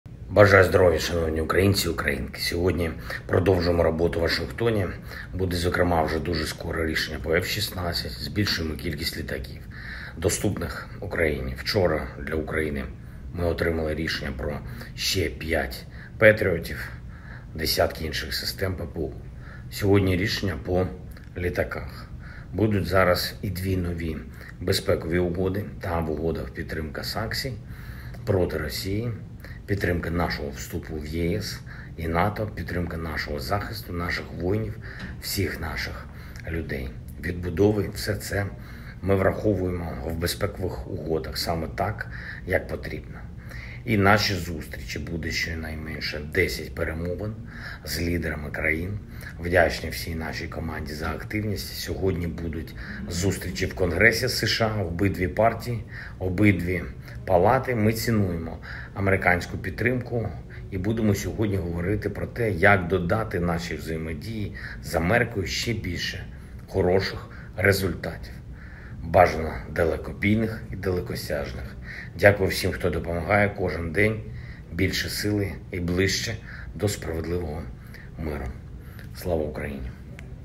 [жестова мова] Звернення Володимира Зеленського щодо робочої поїздки до США
«Сьогодні продовжуємо роботу у Вашингтоні, і буде, зокрема, вже дуже скоро рішення по F-16. Збільшуємо кількість літаків, доступних Україні.

Вчора для України ми отримали рішення про ще 5 «петріотів» і десятки інших систем ППО, сьогодні – рішення по літаках.

Будуть зараз і дві нові безпекові угоди».

Звернення Володимира Зеленського щодо робочої поїздки до США.